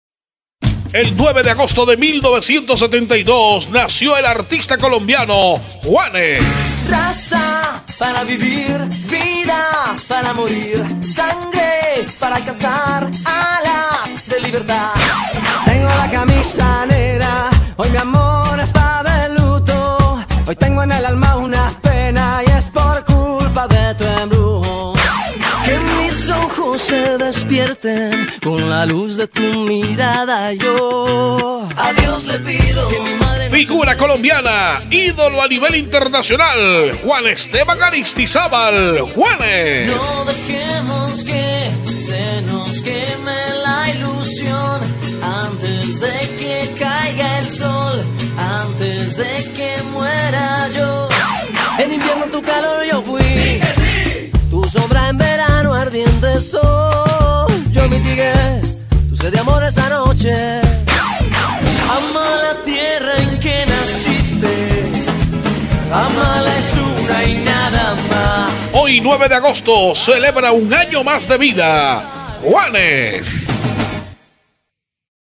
Juanes. Cumpleaños. Agosto 9.
El 9 de agosto de 1972 nació en carolinadelprincipe colombia el artista conocido como @juanes
Desde niño mostró sus dotes como guitarrista, compositor y cantante hasta llegar al lugar de privilegio que ostenta hoy a nivel mundial.
Sus canciones tienen esas raices Colombianas y ese toque internacional que las vuelve Exito.
Feliz Cumpleaños